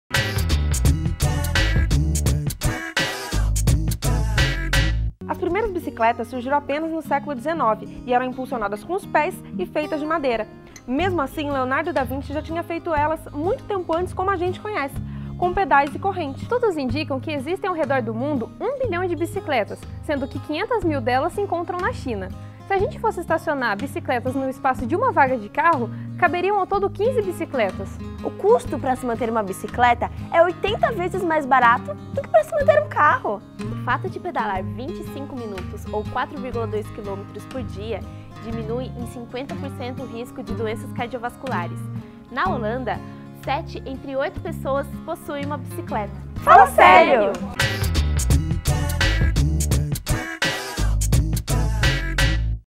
Fala Sério - Bicicleta
O programa de hoje é sobre algo que se aprendemos a usar uma vez nunca mais esquecemos: a bicicleta!

Apresentação: Gabriela Almeida, Louise Guerreiro, Rebeca Miscow e Sabrina Nascimento.
Imagens: Amanda Sanches
Edição: Rebeca Miscow
Direção de Fotografia: Jorge Silva
Coordenação: Fabiana Pupio
Direção Geral: Fernando Moreira